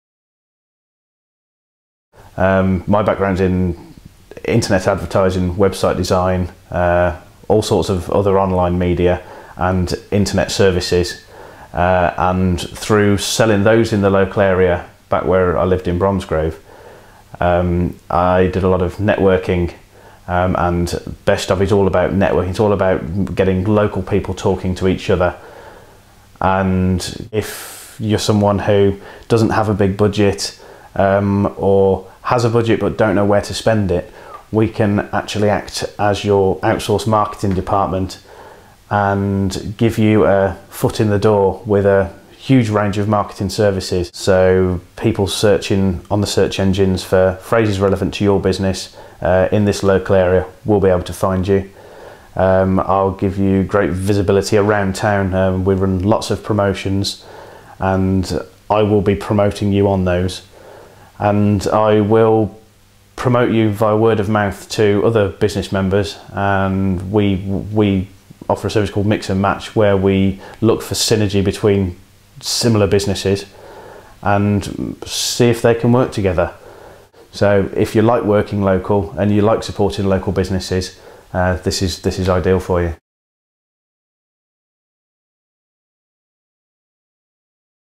Sandy Burt - The Best of Wells
- Sandy Burt tells us about his credentials and some of the many reasons why local businesses in and around the city of Wells should join him at the Best of Wells.